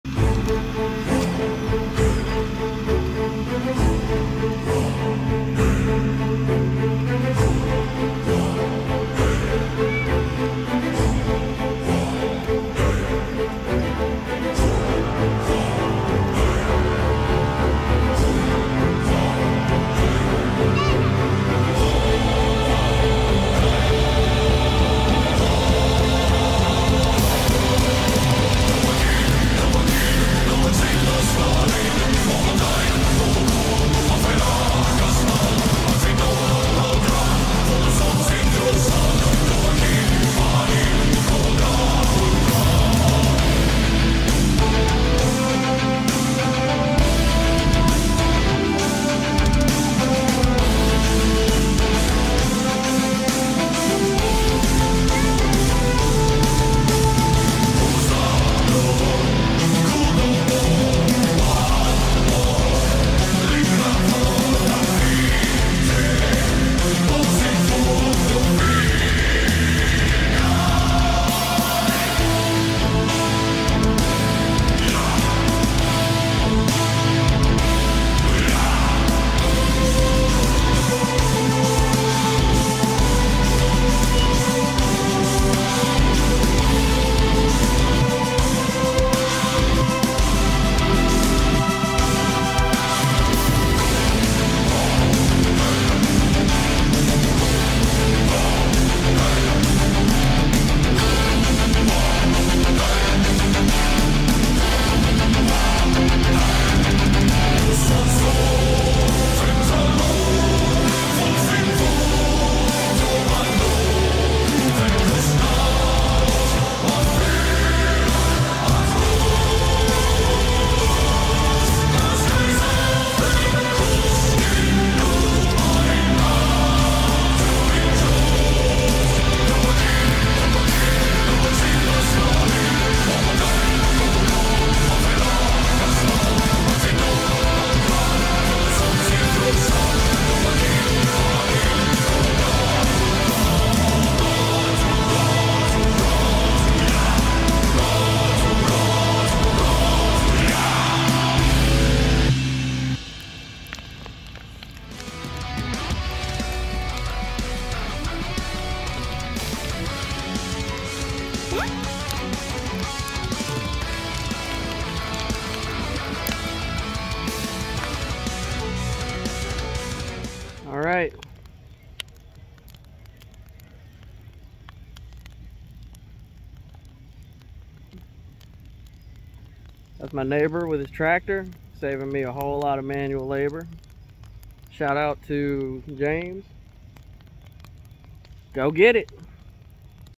EPIC Kubota Tractor!
Best way to load wood onto a fire.